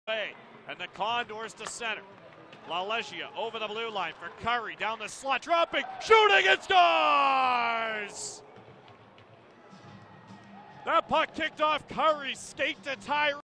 HIGHLIGHT | Rattie 11th of the year
Ty Rattie makes it 2-0.